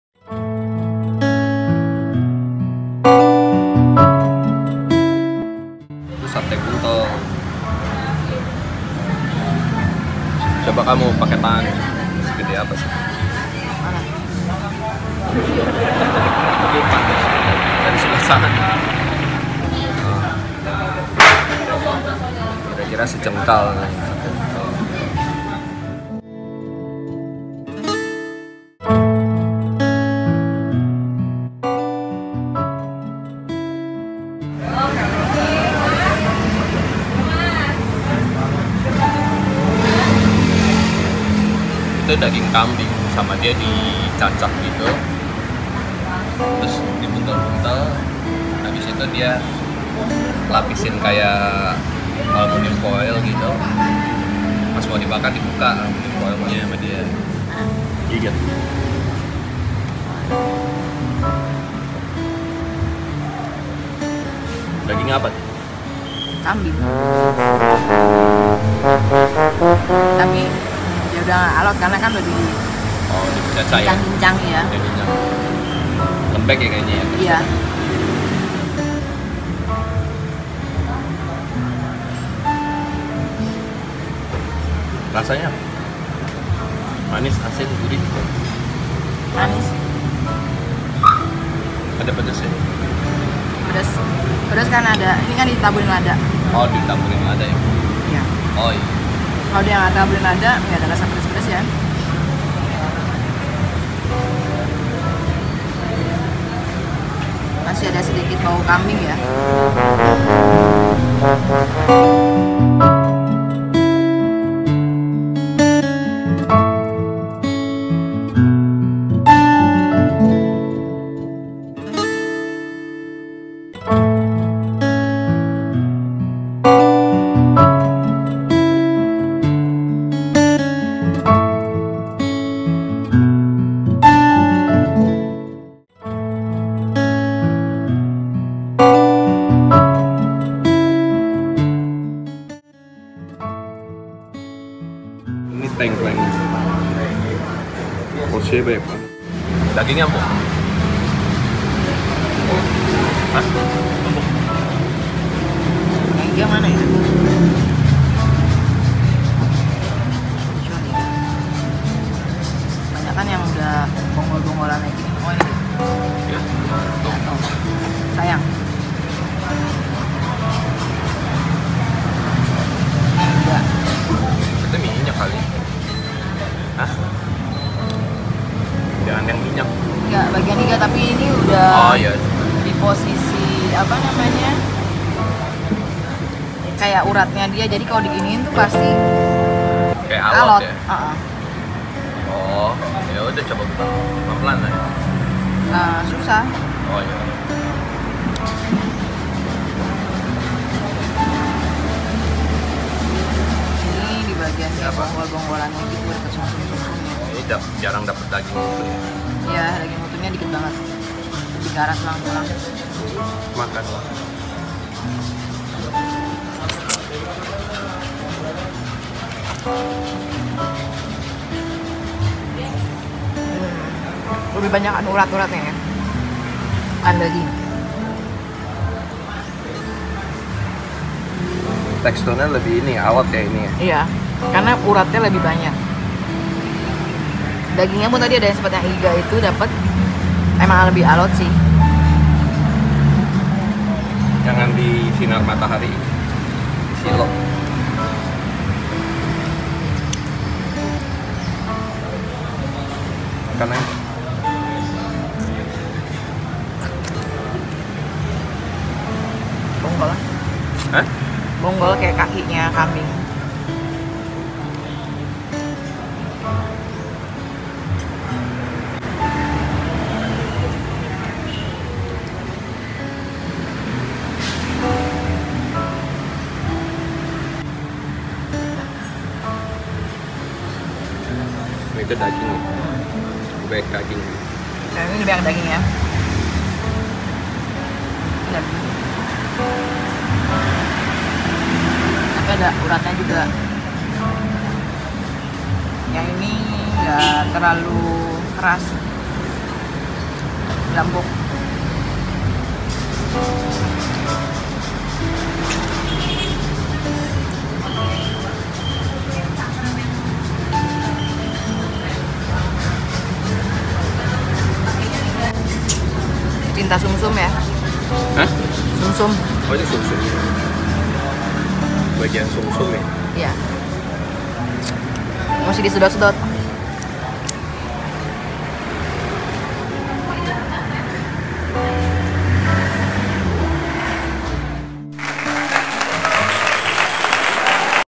1000 PIRING TENGKLENG RICA2 LAKU TERJUAL DLM SEHARI
Destination Kuliner Solo.
Selain terkenal dengan selat dan Timlo Sastro, kota Solo identik dengan Tengkleng dan sate Buntel nya. Paling terkenal ya Tengkleng rica rica Pak Manto.